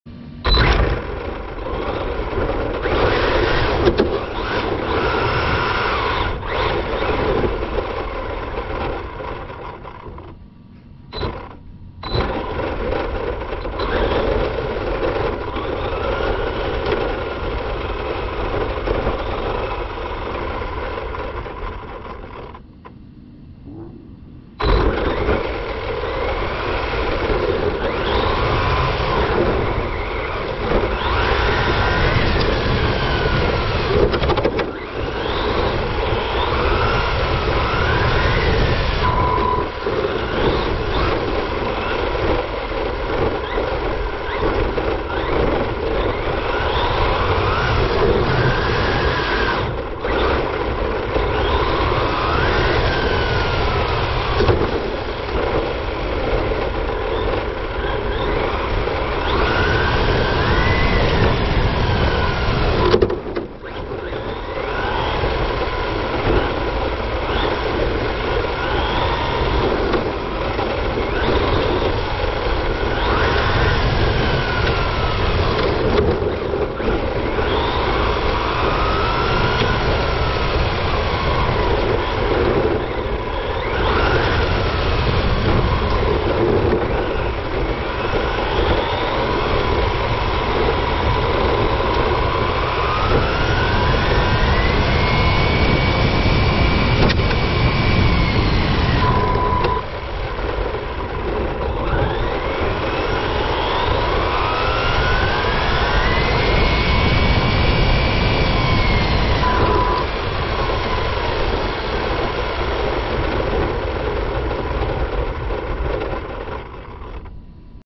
GoPro Max 360 VR Onboard Arrma Vendetta
A (poor) attempt at mounting a GoPro Max on my Arrma Vendetta. It worked fine-ish for this video, but a few minutes after this one ends while filming non-360 footage, the adhesive failed and sent the camera tumbling.